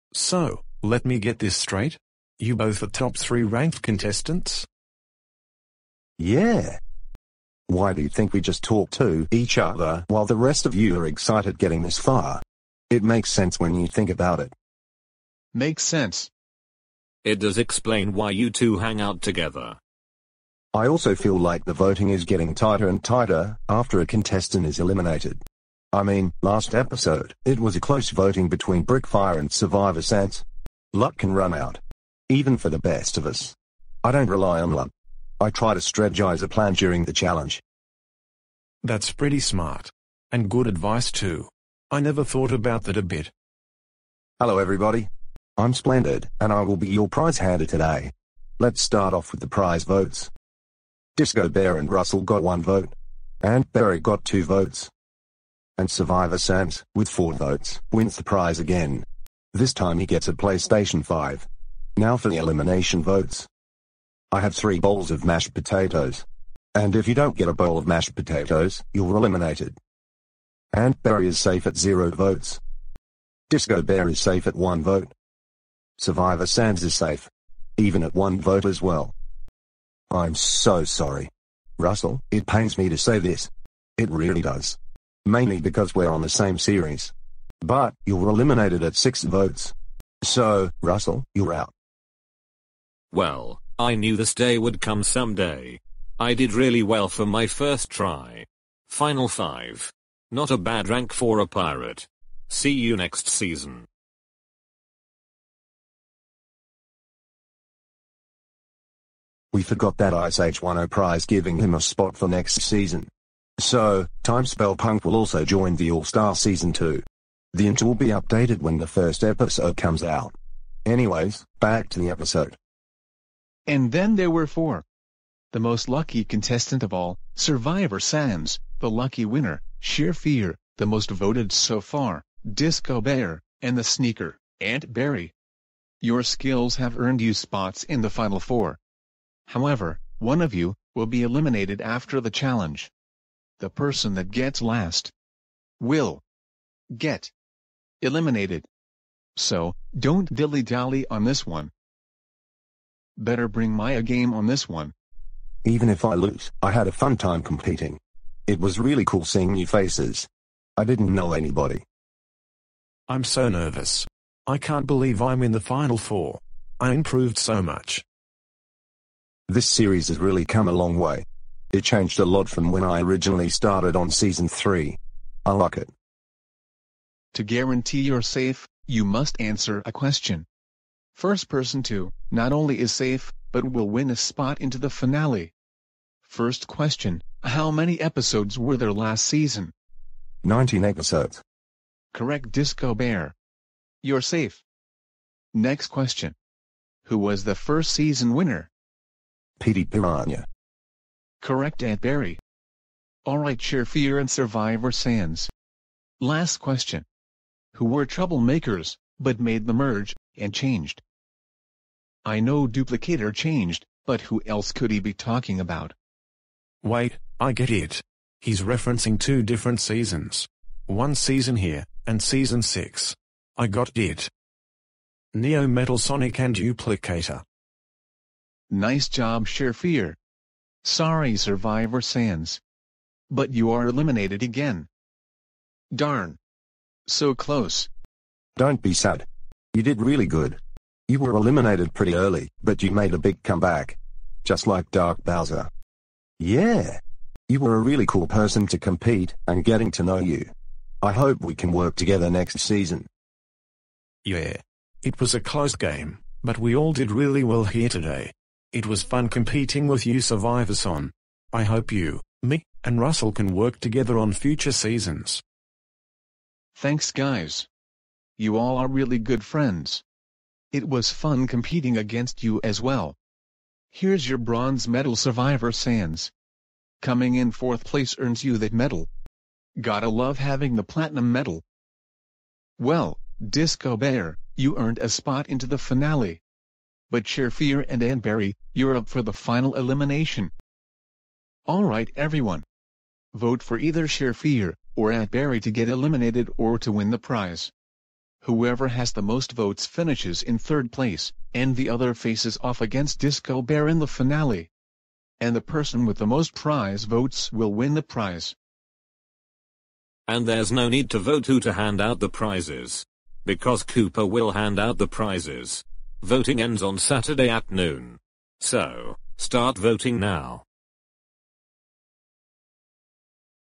Character Elimination: Rookies Vs Veterans: Episode 17: The Big Question
The final 4 are put at high stakes in this challenge, each question is asked from this season and past season! And it's at a big risk too!